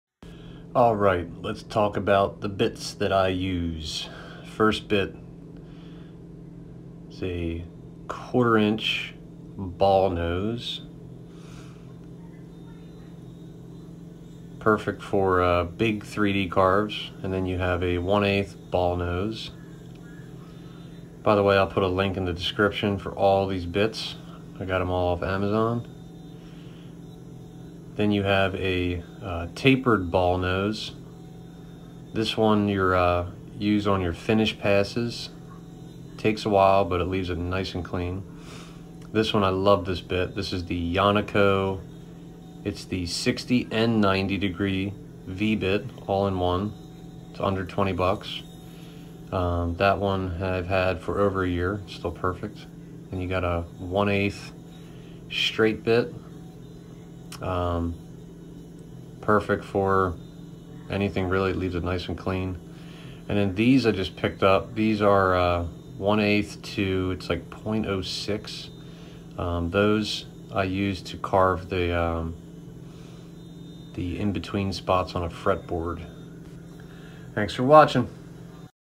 My most common used cnc bits
Bits I use

Tapered Ballnose for 3d finishing

0.6mm Carbide endmill for Fret Board

1/8 Straight bit

1/8 ballnose Bit

1 inch spoilboard resurfacing bit

Wallmount bottle openers

Forstner Bits